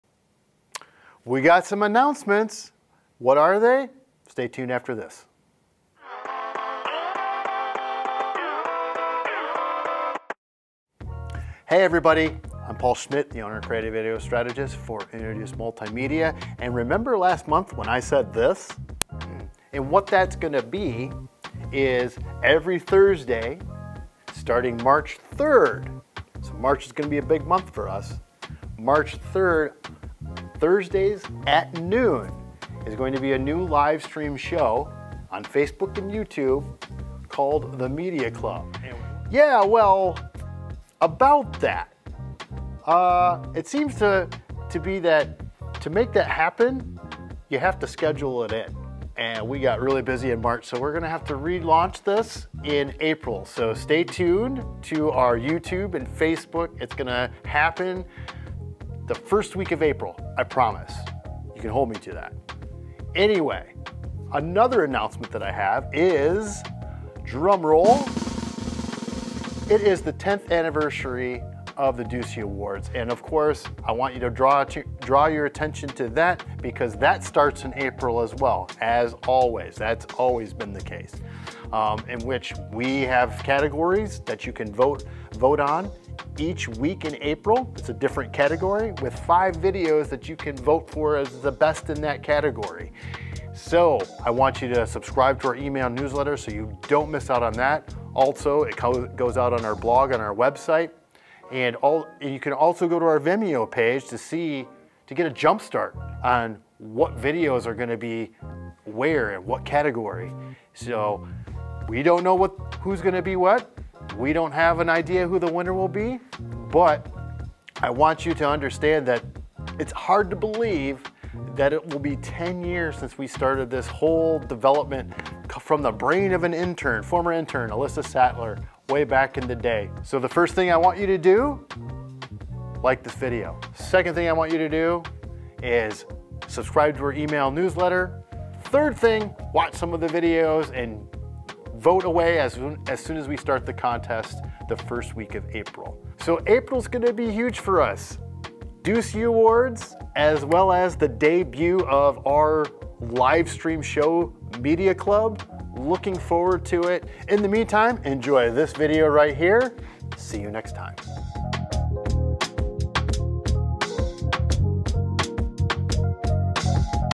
Announcements Part 2 - Redux - Electric Boogaloo - The Return
April is going to be Huge!! Starting with the very first week, we will be kicking off the  10TH ANNUAL Deucey Awards! The entire month of April will be dedicated to these awards.

A new category will start each week, with the ability to vote on them on our website. In the meantime, our email newsletter will shift from Thursdays to Mondays to reflect that.

Can't wait to see how this goes.